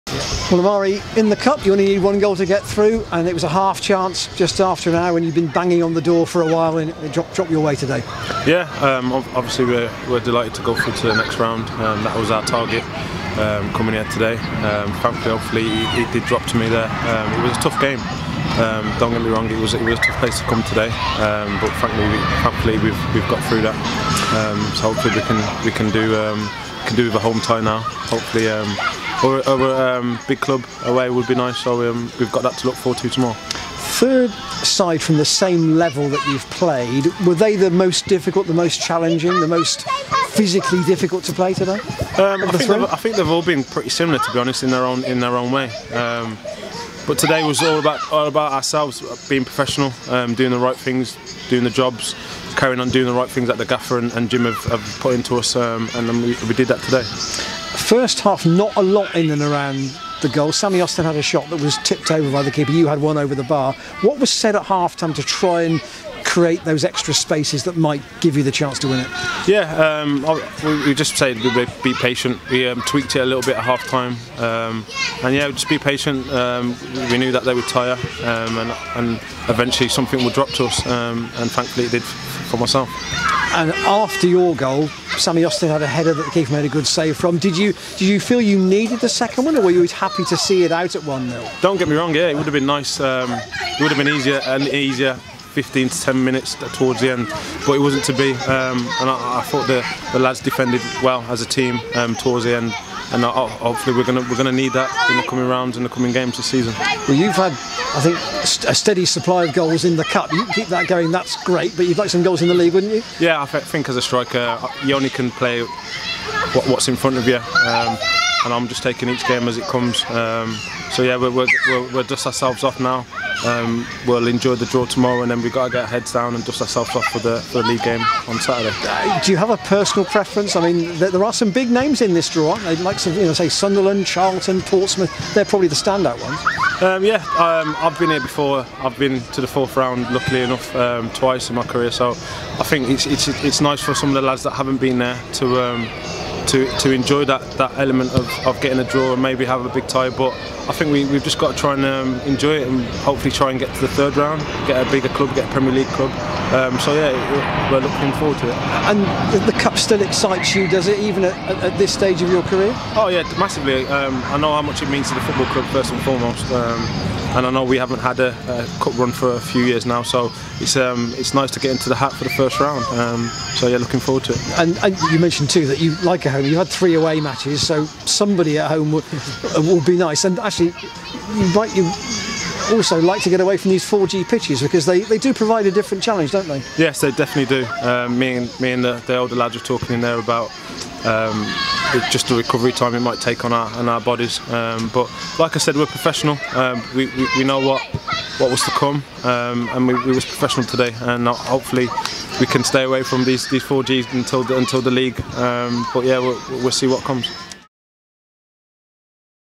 Amari Morgan-Smith post-match: Bedfont Sports 0-1 Harriers 16/10/21
Amari Morgan-Smith talks following Harriers' victory to reach the First Round Proper of the Emirates FA Cup with victory against Bedfont Sports.